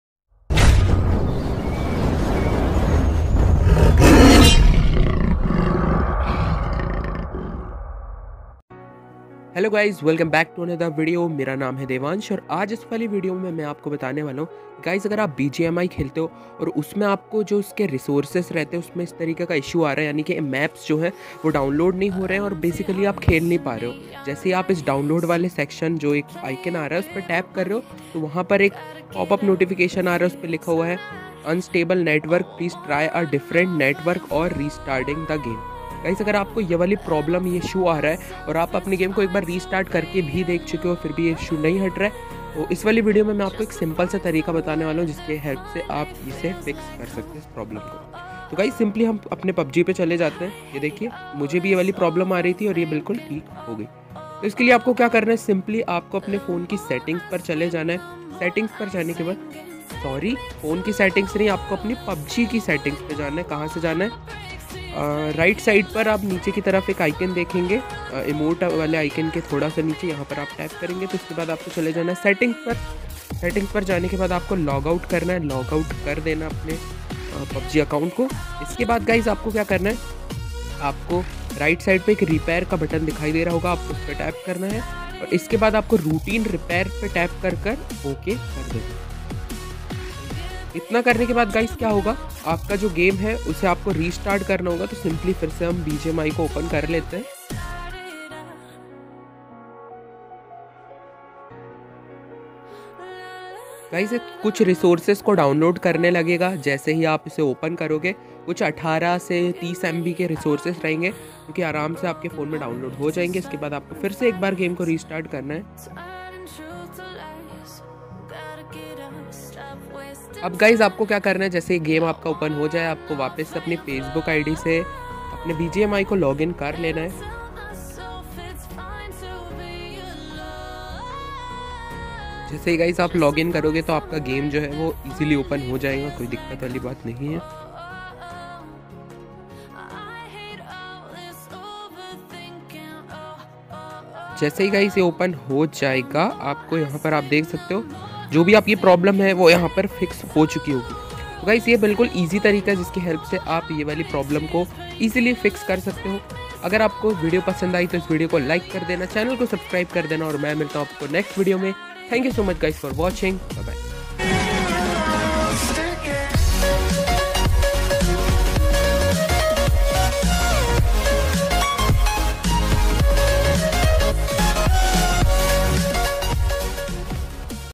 HOW TO FIX BGMI Unstable network Please try a different network or restarting game Problem Solved
Welcome back guys to the new video and in this video, I'll show you Hello Guys, Welcome back to another video and In this video i will show you How to fix bgmi Unstable network. Please try a different network or restarting the game.